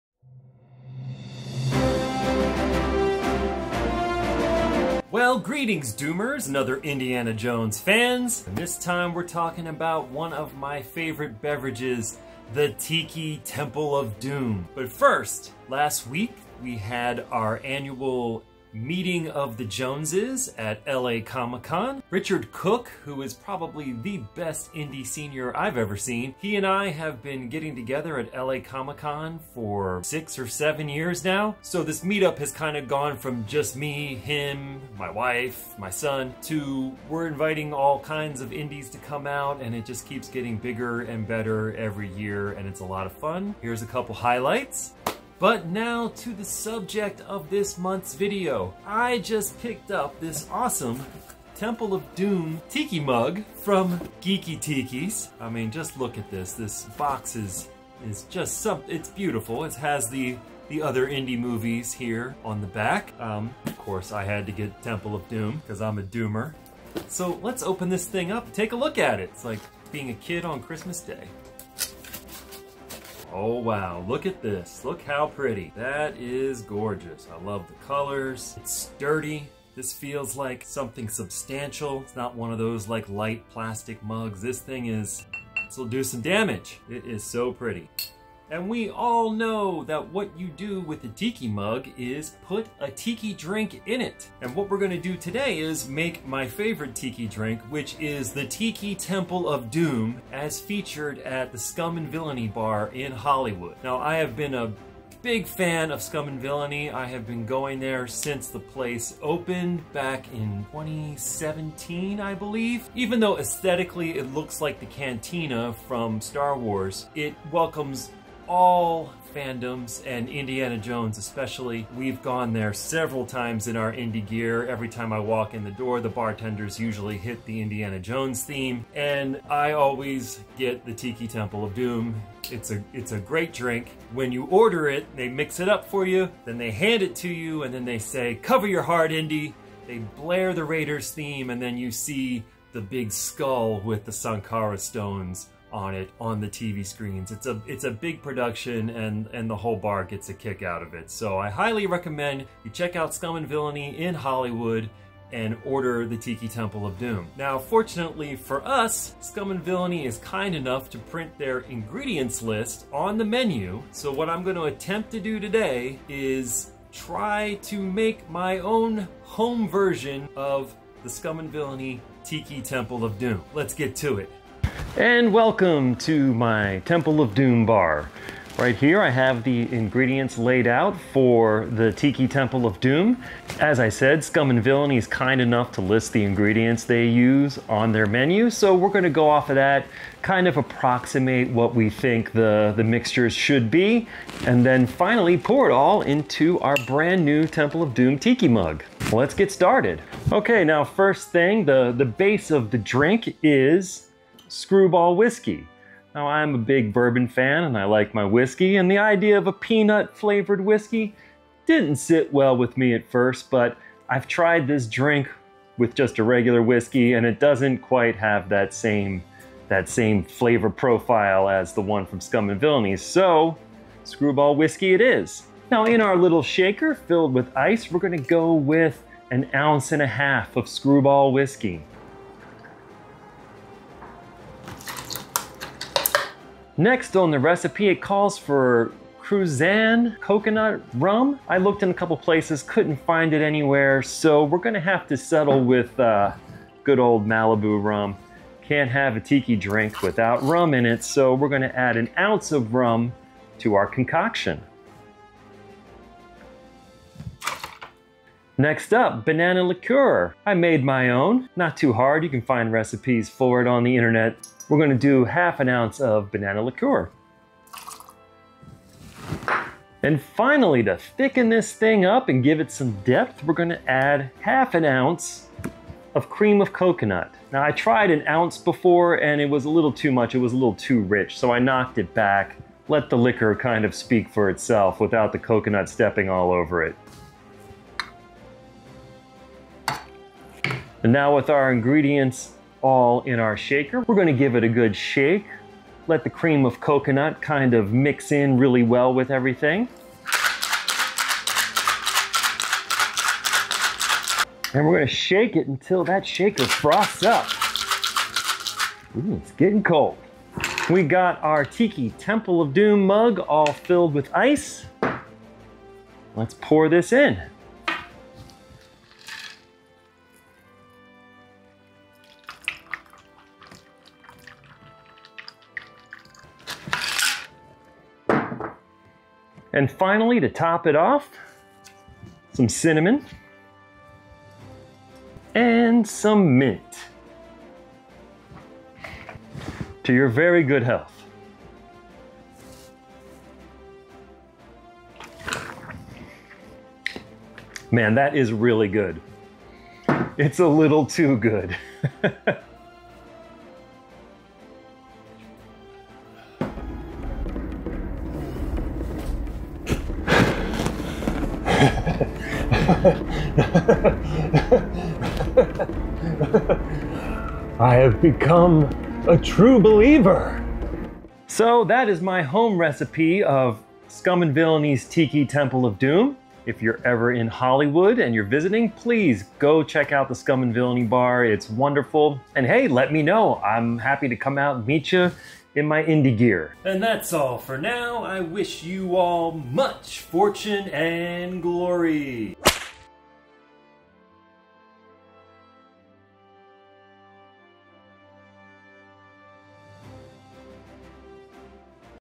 Attempting to Make a Tiki Temple of Doom!
I attempt to recreate the Scum & Villainy bar's Tiki Temple of Doom cocktail, and pour it into my new Geeki Tiki Temple of Doom mug!